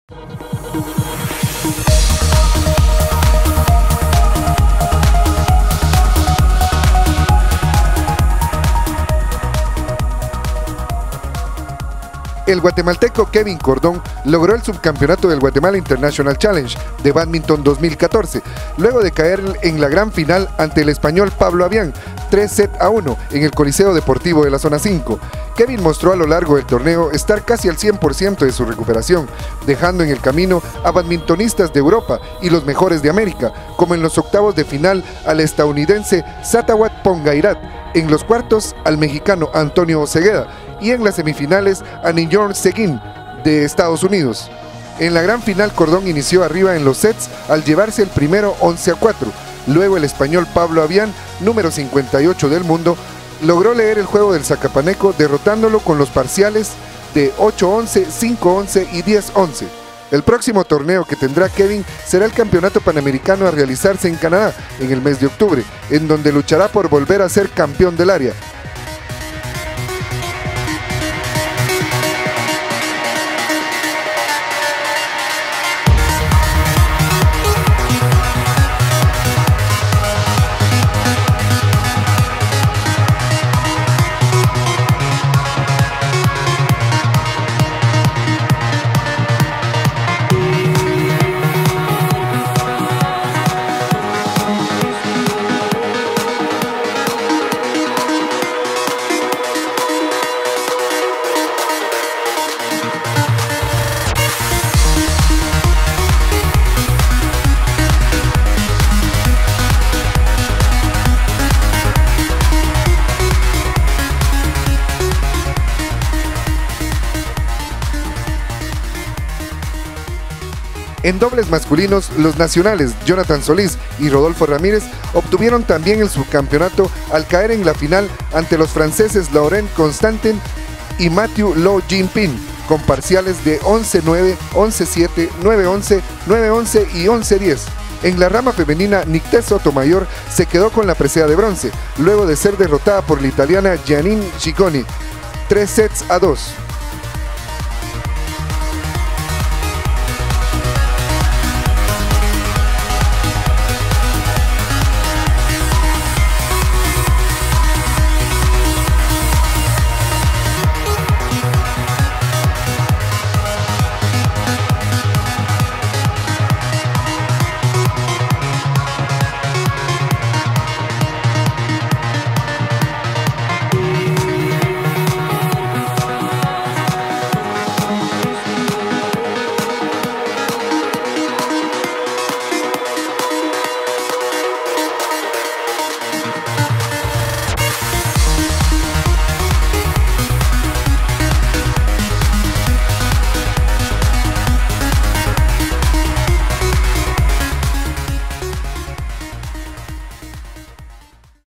Guatemala Internacional Challenge Badminton 2014
El guatemalteco Kevin Cordón logró el subcampeonato del Guatemala Internacional Challenge de Bádminton 2014, luego de caer en la gran final ante el español Pablo Abian 3 sets a 1 en el Coliseo Deportivo de la Zona 5.

En dobles masculinos:

Los nacionales Jonathan Solís y Rodolfo Ramírez obtuvieron también el subcampeonato al caer en la final ante los franceses Laurent Constantin y Matthieu Lo Ying Ping con parciales de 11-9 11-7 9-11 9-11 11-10.

En la rama femenina

Nikté Sotomayor se quedó con la presea de bronce, luego de ser derrotada por la italiana Jeanine Cicognini 3 sets a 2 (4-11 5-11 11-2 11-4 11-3)

Cámara: Fernando Hanser
Edición: Antonio Duque